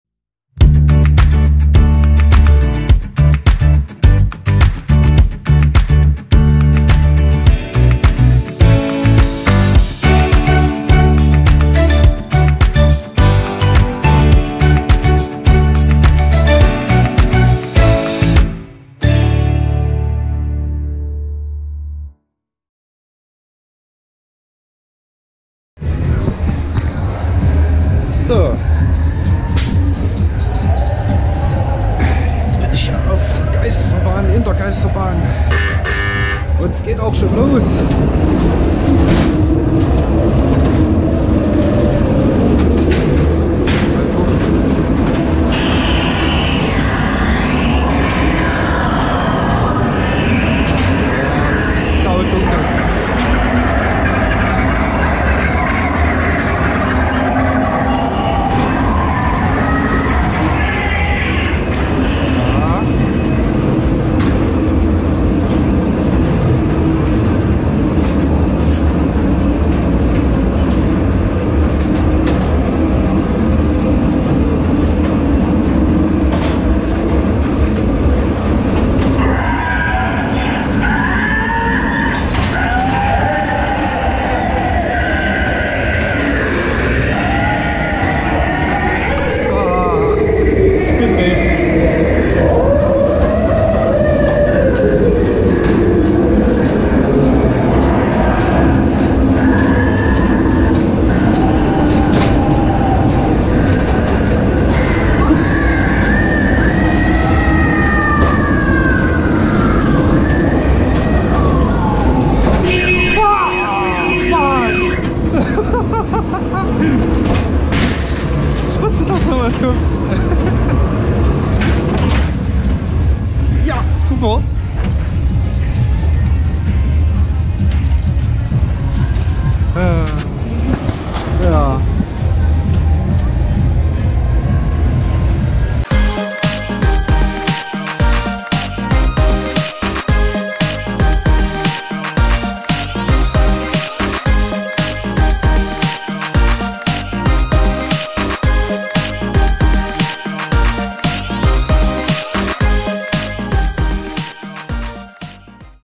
Fun House - Onride - Matejska Pout 2018 / Lunapark Prag
Fun House (ehemalig Horror Castle) ist eine Geisterbahn. Aufgenommen auf der Matejska Pout 2018 / Lunapark Prag.

Die Geisterbahn ist zwar schon älter, dafür sind die Effekte aber ganz ok. Die Beleuchtung der Effekte/Figuren war leider zu dunkel. Das Horn am Ende ist echt fies XD